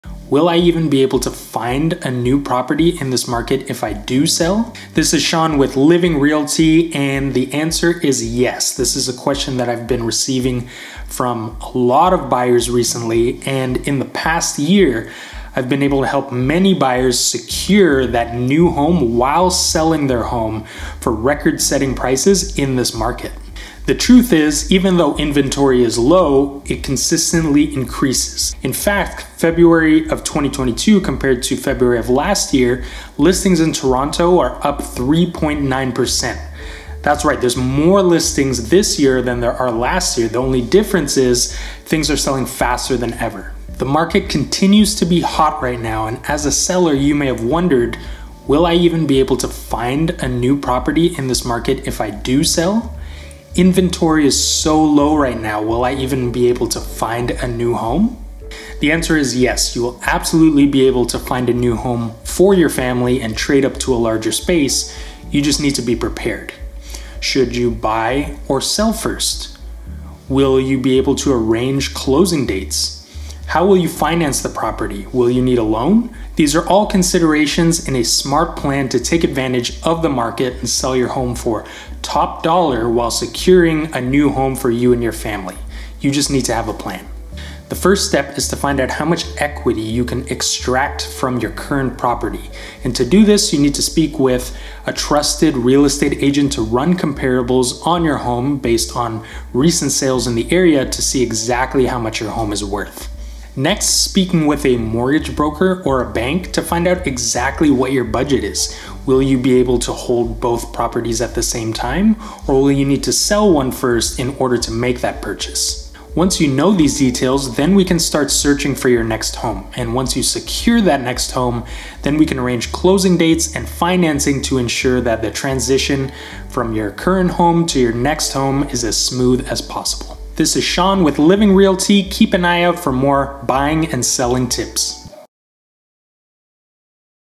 Planning To Sell My House // How Much House Will I Get Approved For?
I'm planning to sell my house and as a seller, you may have wondered How Much House Will I Get Approved For?  With these Real Estate Prices 2022, Should I be Buying and Selling A House At The Same Time? Will I even find a house if I do sell?

I've been able to help many buyers in the 2021 and 2022 Toronto Real Estate Market find their next home while selling their home for record setting prices.